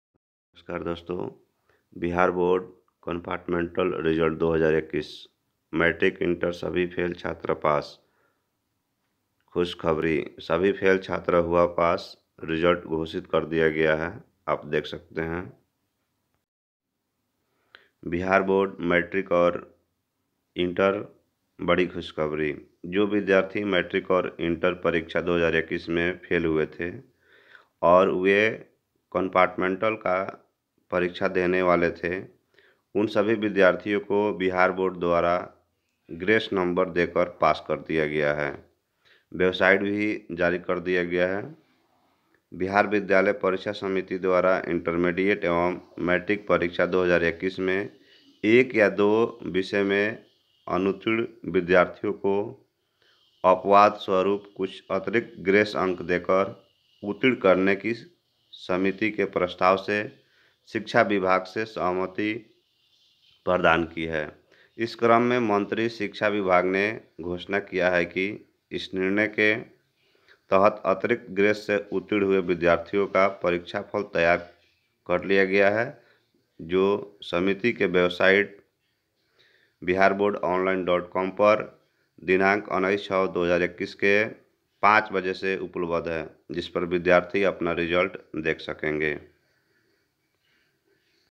bihar board compartmental result 2021/बिहार बोर्ड मैट्रिक इंटर सभी फेल छात्र हुए पास
बिहार बोर्ड कंपार्टमेंटल रिजल्ट 2021// बिहार बोर्ड मैट्रिक इंटर सभी फेल छात्र हुए पास

link